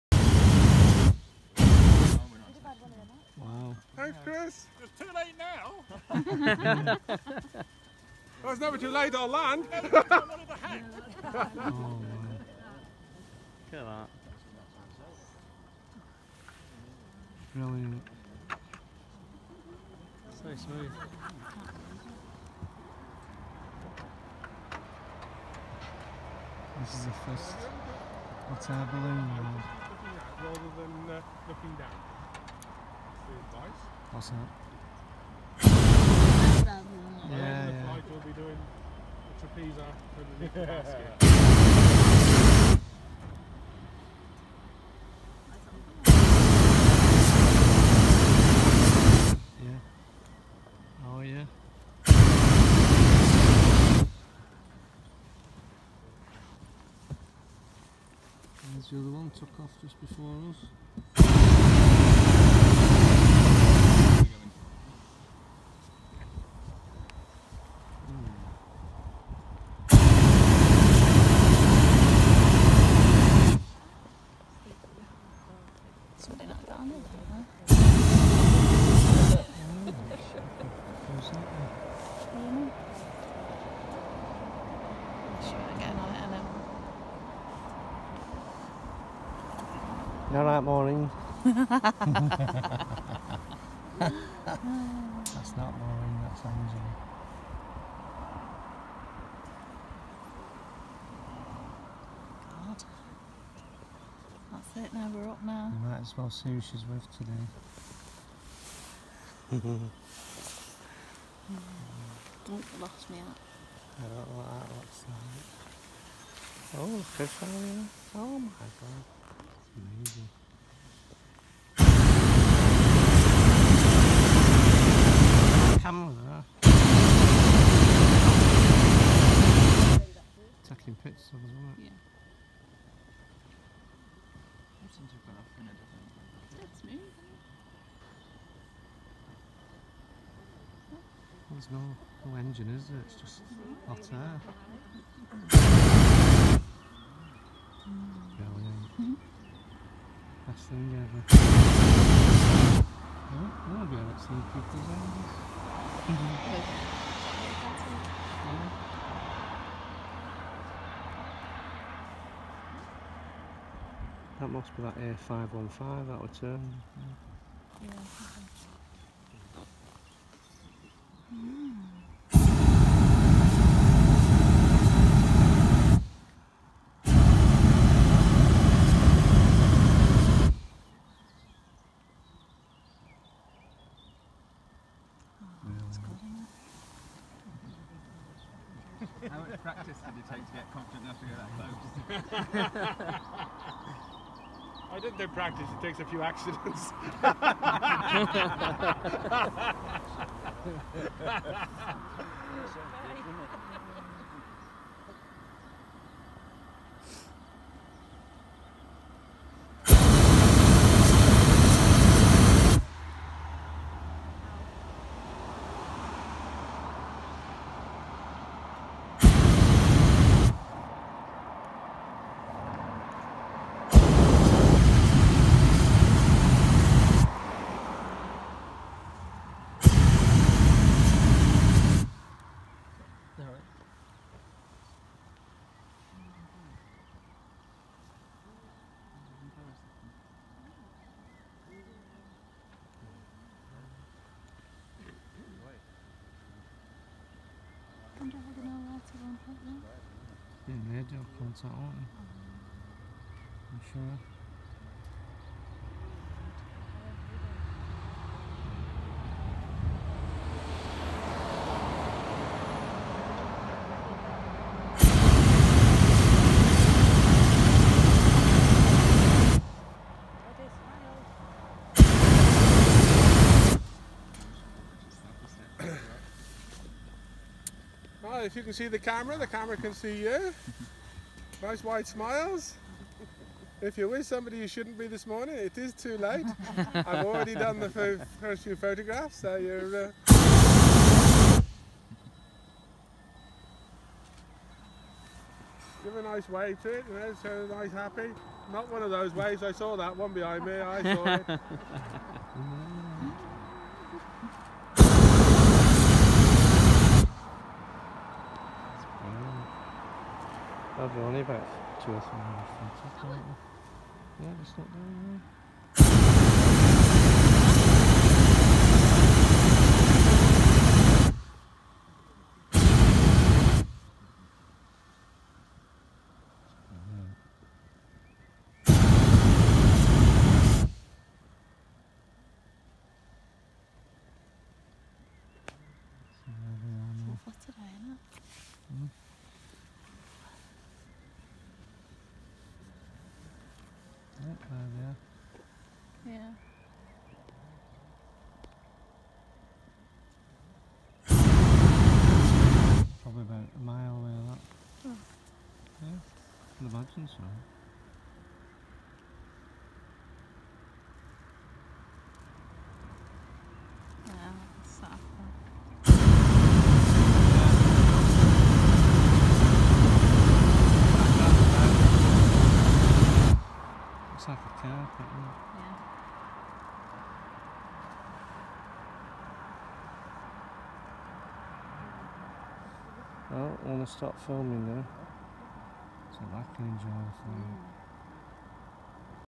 Virgin hot air balloon flight June 2018
Flight from Tissington near Ashbourne. Wonderful views!